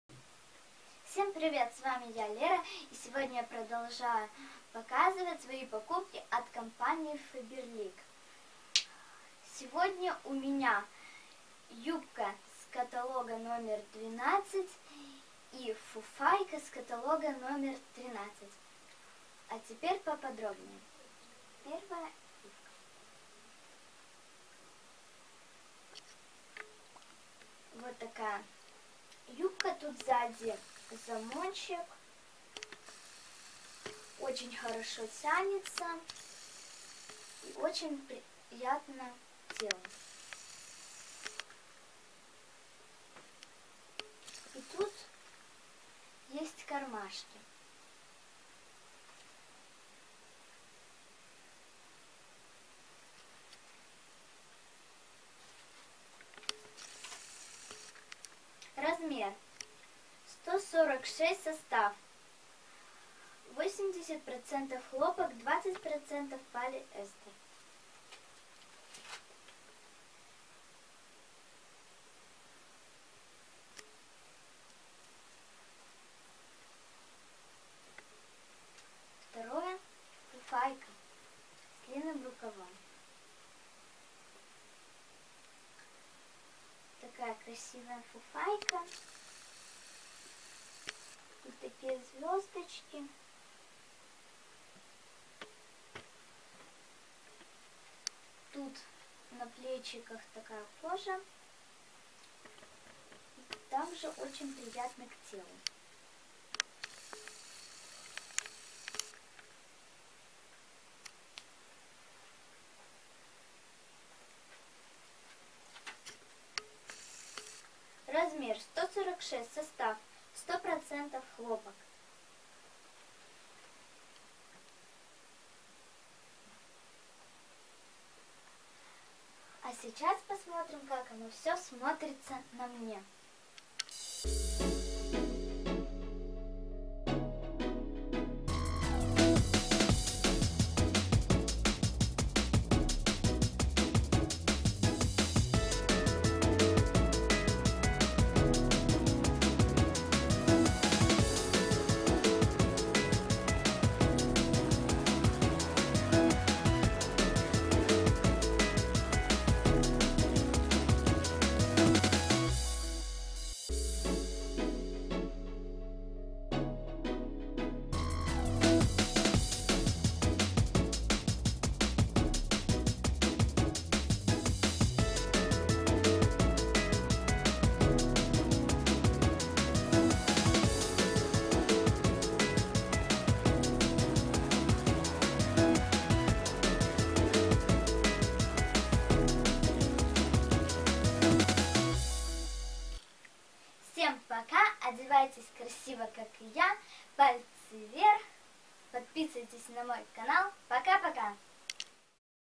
Одежда от ФАБЕРЛИК из каталогов №12 и №13, 2016 года.
Видеообзор. Одежда для девочек от ФАБЕРЛИК из каталогов №12 и №13 2016 года (юбка плюс фуфайка(футболка) с длинным рукавом). Красивая и качественная одежда от faberlik. Показываю, расказываю, примеряю.